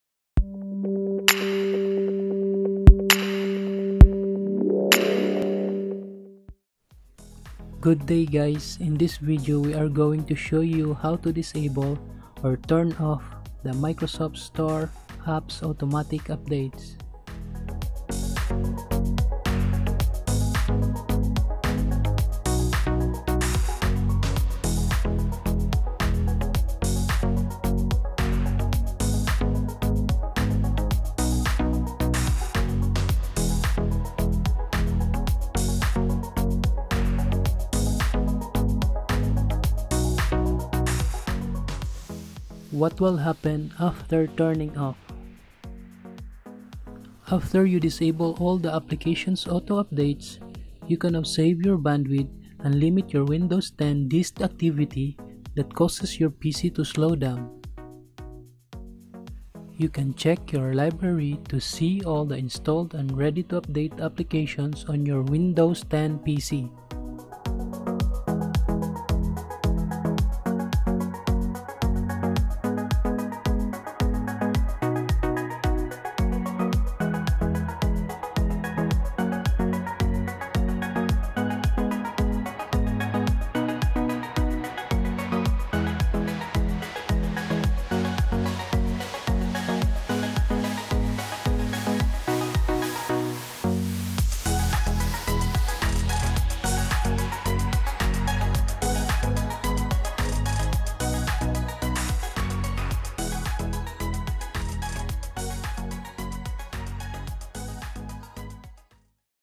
Disable or Turn Off Microsoft Store Apps Auto Updates in Windows 10
Here's how to disable or Turn Off Windows 10 Microsoft Store app automatic updates. This can save your data or bandwidth, limit background disk activity and other app related processes on your PC.